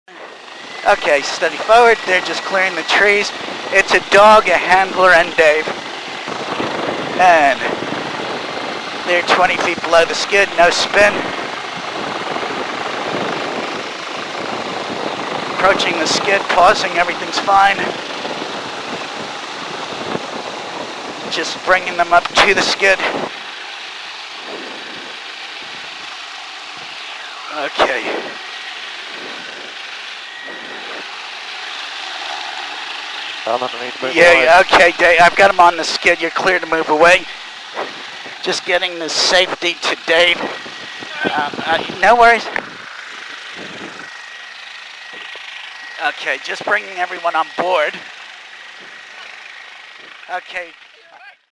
Life Flight winches Police dog and handler into Westpac Rescue Helicopter during SAR op
Sometimes we fly dogs too! This week we winched a very well-behaved Police dog and his handler into our Westpac Rescue Helicopter.

Searchers located two lost hikers at 5am after a long night, then we winched the hikers and Police officers out of the bush. Fortunately the third hiker made his own way out on foot unharmed.

A great result with all three safe and sound.